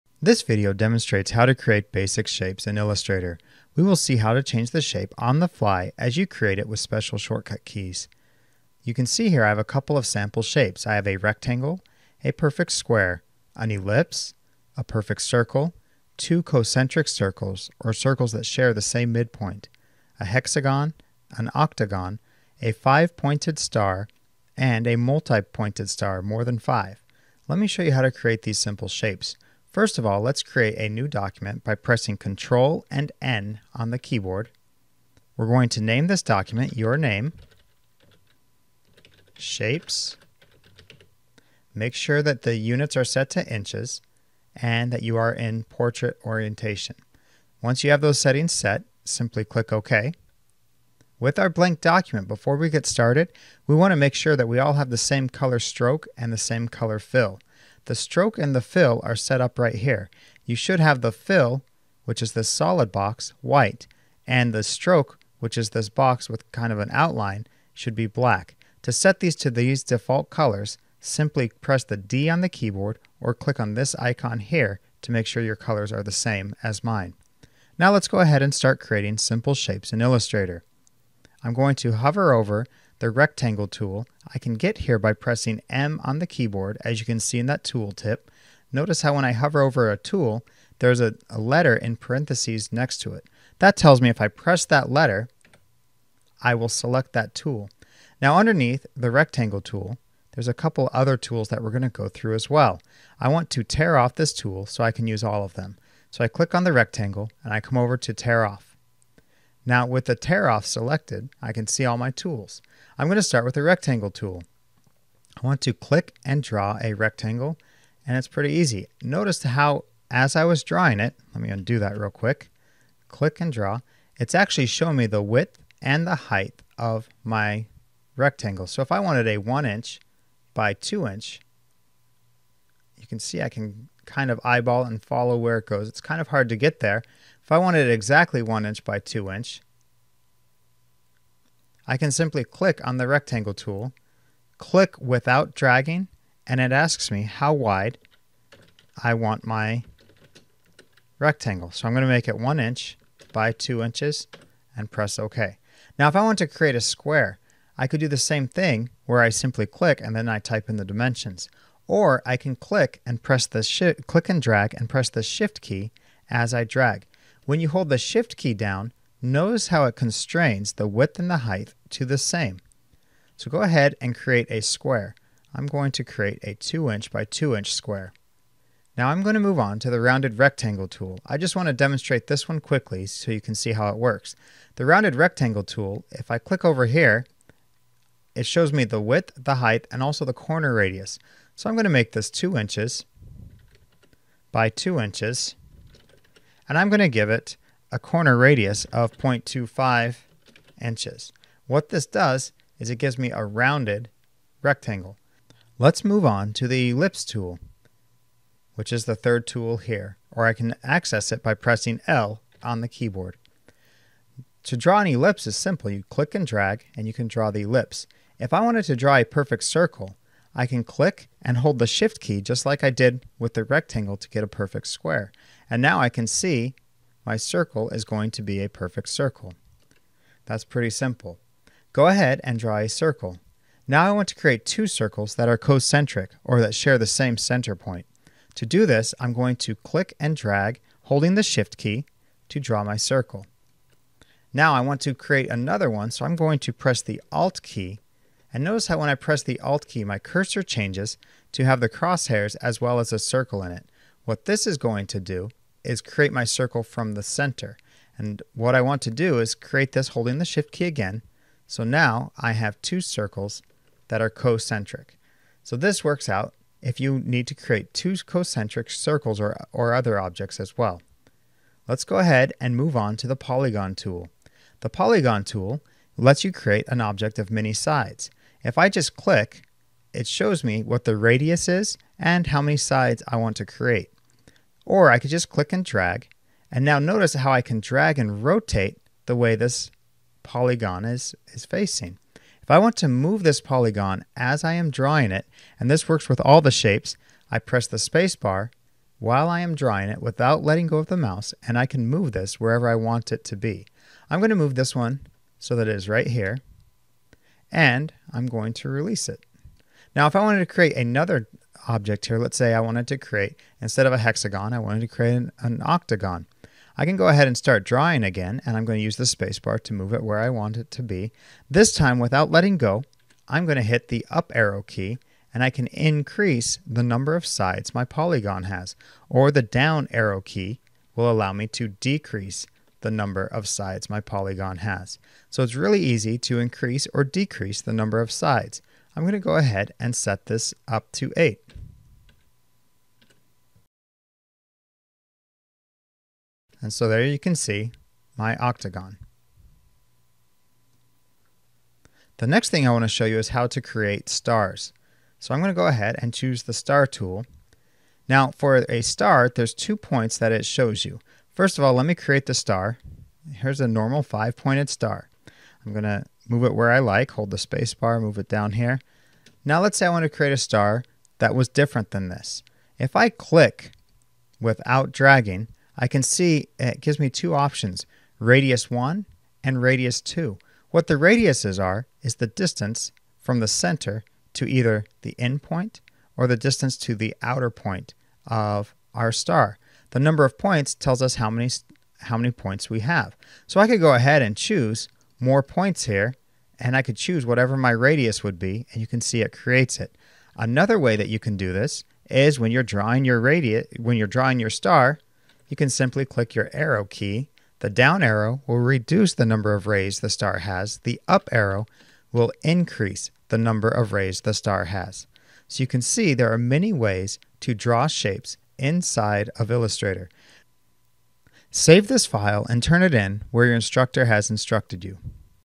Adobe Illustrator Basics: Shape Tool-- How to create basic Shapes including shortcut keys
This is part of a series of videos created to accompany a Jr High or High computer class, but really they are great for anyone who wants to learn the basics of Adobe Illustrator. The videos teach basics of how to use the tools in Adobe illustrator. The videos are designed for a novice illustrator user, but they may help a self taught illustrator user use the tools more efficiently. This simple video instructions are designed to aid in Project based learning (PBL). After students learn the tool basics they are given a creative project that incorporates the tools in the videos. Some videos build upon previous videos or reference a file--but they should be clear enough without seeing the referenced video or having the file.